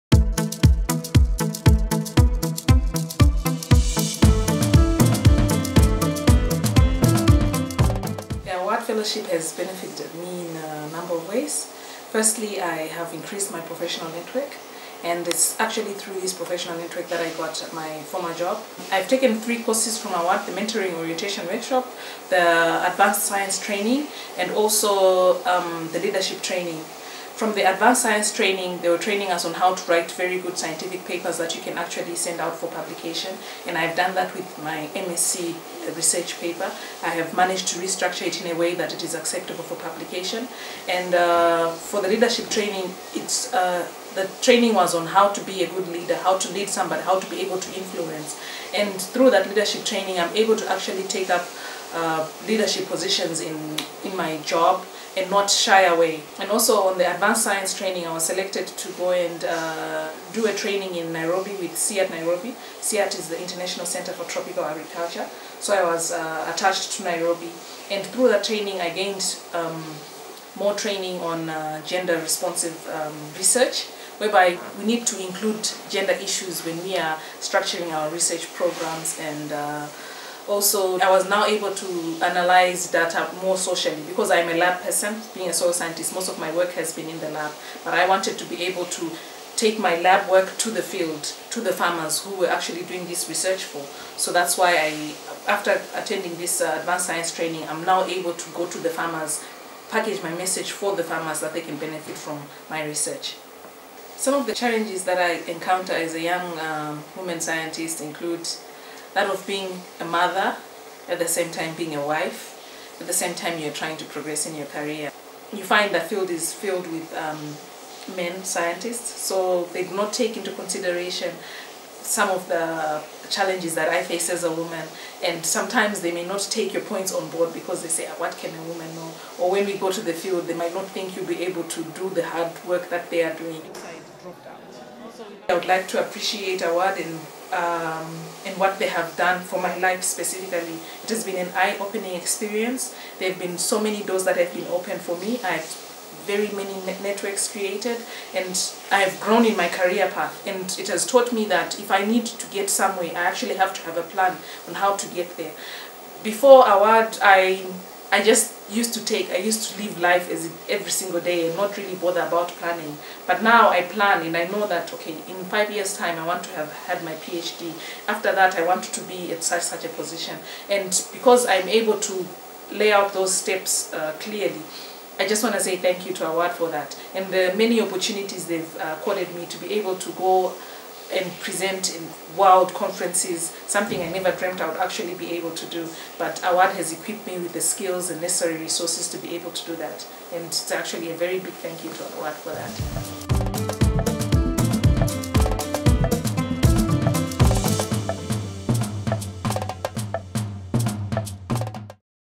Unearthing a basket of opportunities - Caroline Sibusisiwe
After winning the AWARD Advanced Science Training placement, Caroline Sibusiswe, a soil scientist working mainly in laboratories, repackaged her research to focus mainly on farmers.
This 2013 Fellow, is content that AWARD taught her the importance of planning and encouraged her to indulge in gender responsive research, a lesson she deems invaluable.